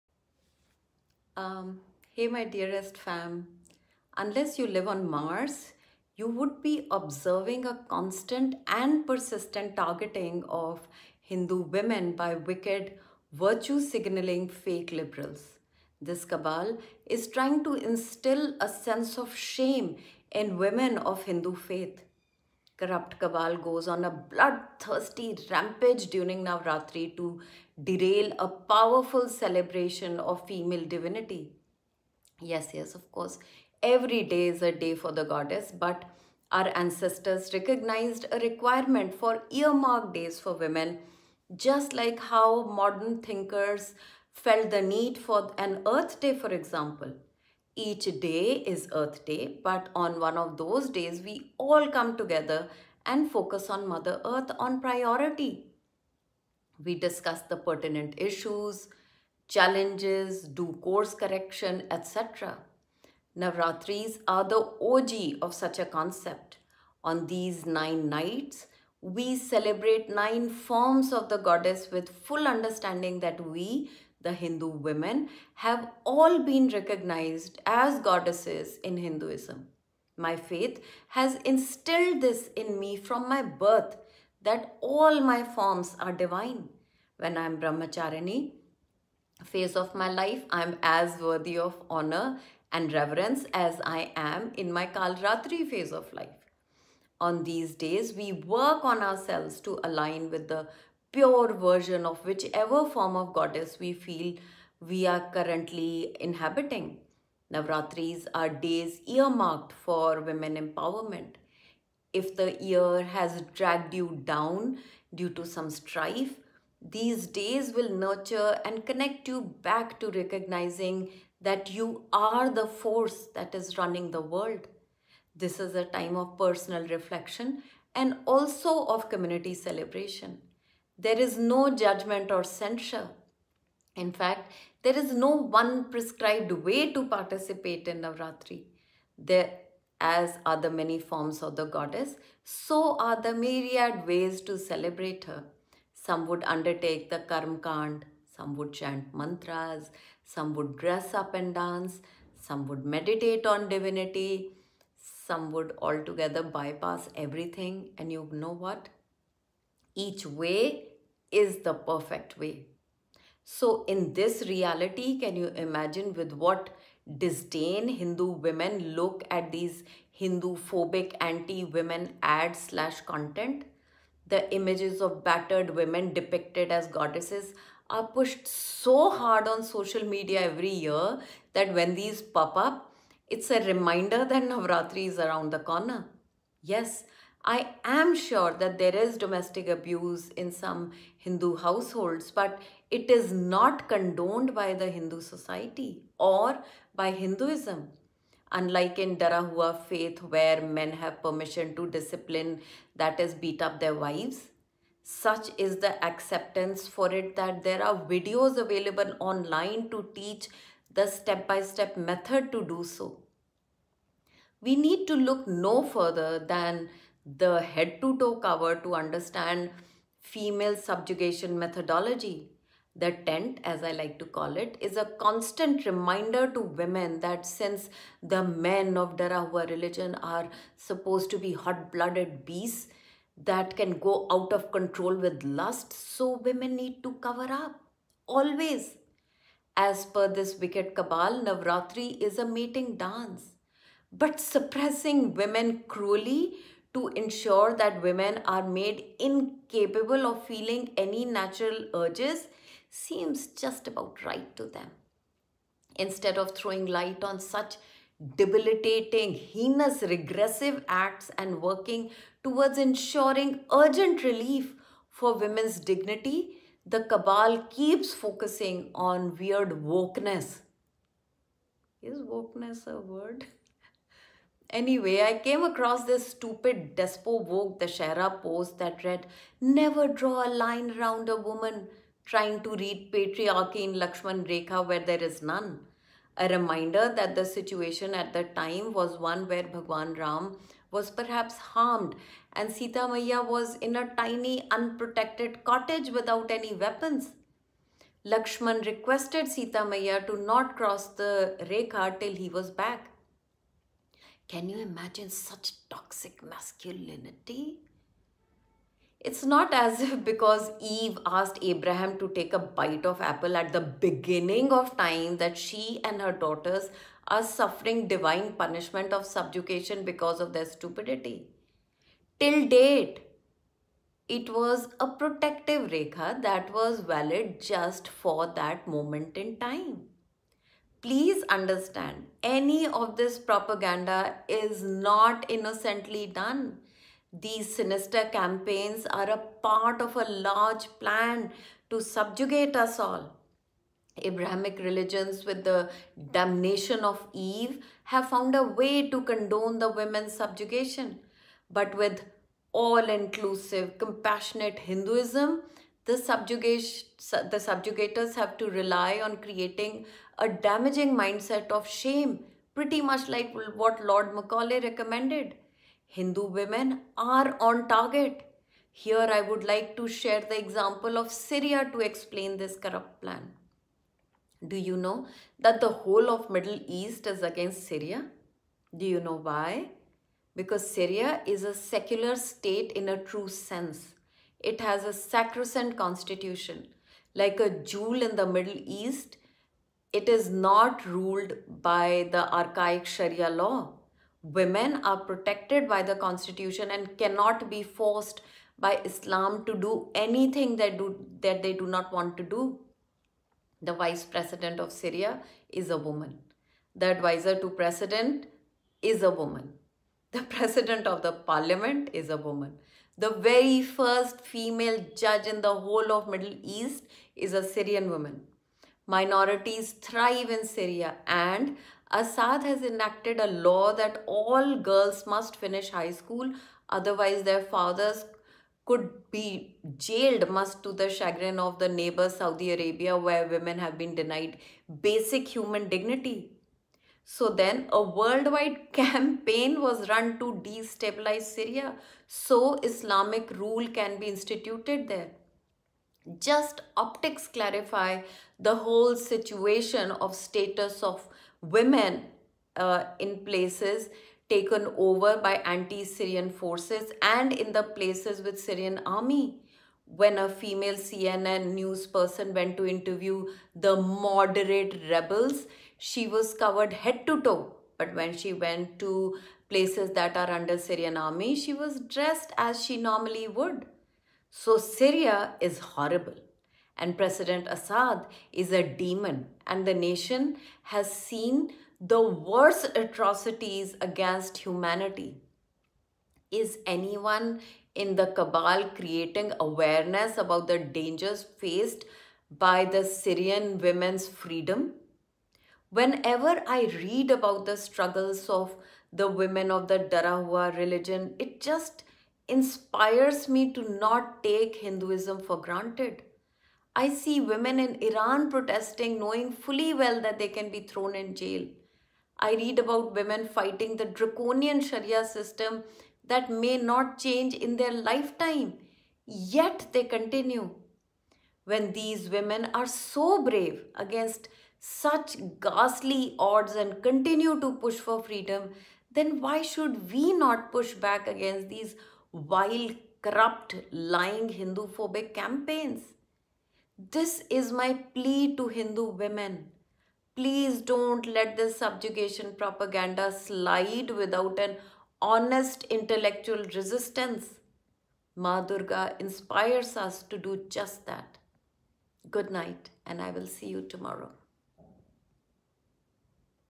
☕️The Daily Nightcap.☕️ A Hindu woman's pushback.
The wicked cabal is out to shame Hindus, this time at the pretext of Navratri.
Hinduism inspires me to be a compassionate evolving being. Hinduism empowers me with the knowledge that every woman is a goddess. I will not sit quietly and let this beautiful faith get pulled down by this hyena-like cabal.
I will pushback.